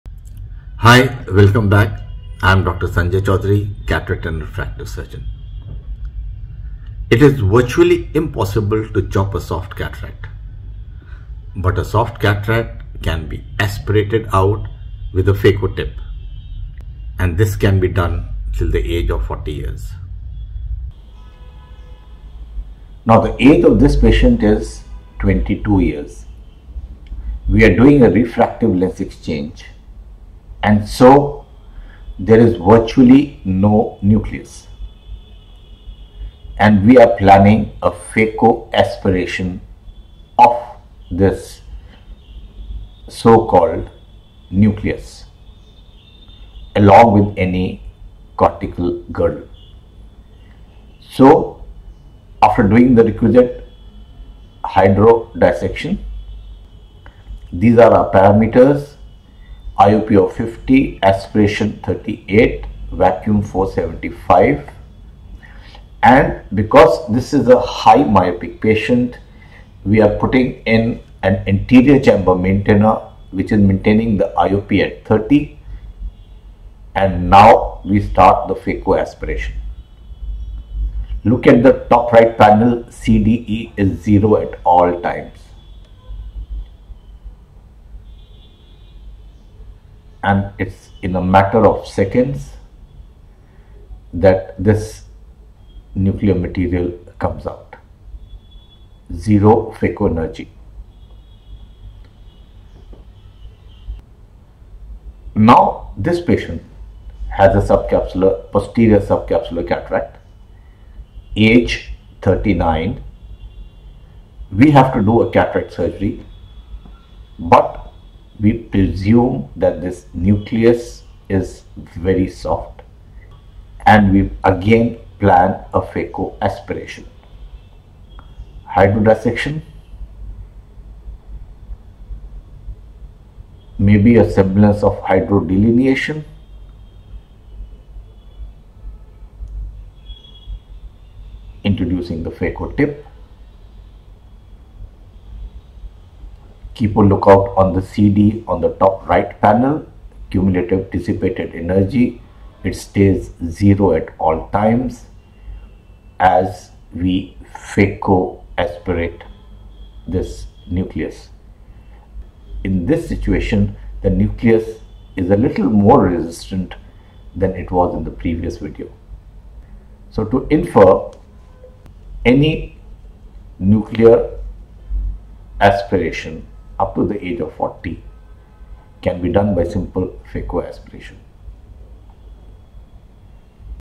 Phaco Aspiration (Zero Phaco Energy) in a Soft Cataract
In this video, Dr. Sanjay Chaudhary demonstrates How Phaco Aspiration (Zero Phaco Energy) can be done in a Soft Cataract in patients up to the age of 40 years.

ABOUT DR. SANJAY CHAUDHARY
(Founder of Eye7 Group of Eye Hospitals, Delhi-NCR)